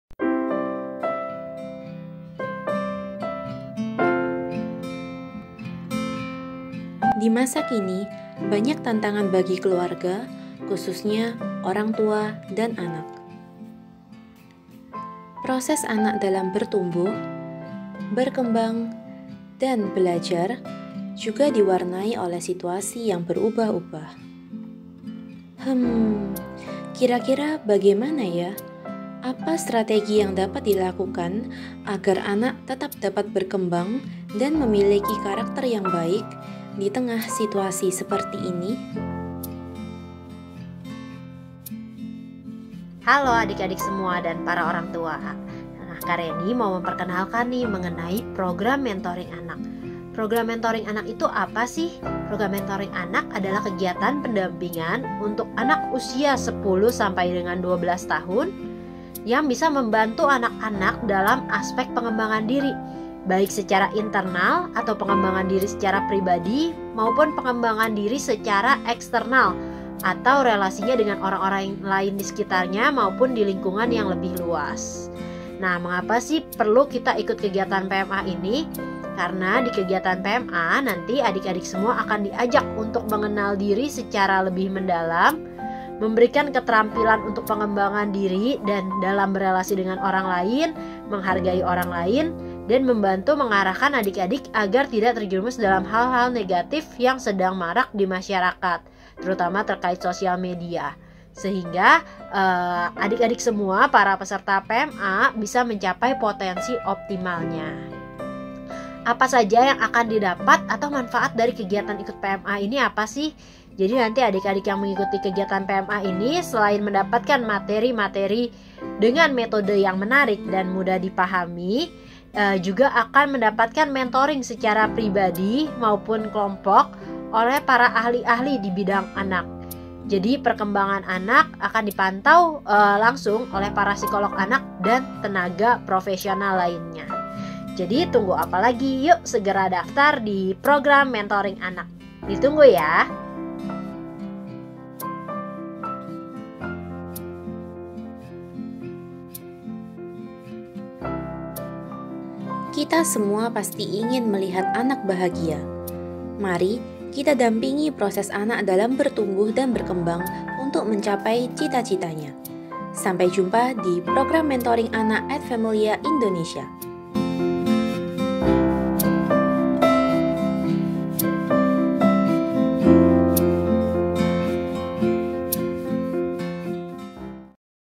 Program Mentoring Anak
Hai Sahabat AFI!
Program Mentoring Anak (PMA) hadir kembali!

“Let the child be the scriptwriter, the director and the actor in his own play.”
-Magda Gerber-

Program Mentoring Anak adalah program khusus dari Ad Familia Indonesia untuk anak usia 10-12 tahun. Dengan didampingi oleh psikolog dan konselor AFI sebagai mentor, anak akan dibantu untuk mengenal dirinya dan menyesuaikan diri agar tangguh menghadapi masa perkembangannya saat ini. Program ini dilaksanakan dalam 10 sesi

Waktu: Setiap Jumat pukul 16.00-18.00 WIB mulai 11 Maret 2022 sampai dengan 27 Mei 2022
Opening: Sabtu, 5 Maret 2022
Closing: Sabtu, 4 Juni 2022
(Pelaksanaan via Zoom)

atau silahkan scan barcode pada flyer. Pendaftaran paling lambat tanggal 25 Februari 2022

Yuk simak infonya lebih lanjut dalam flyer! Apabila ada pertanyaan, silahkan menghubungi Operasional AFI di WA: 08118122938 (chat only)

Selamat mencintai & selamat berkarya!
Terima kasih dan sampai jumpa!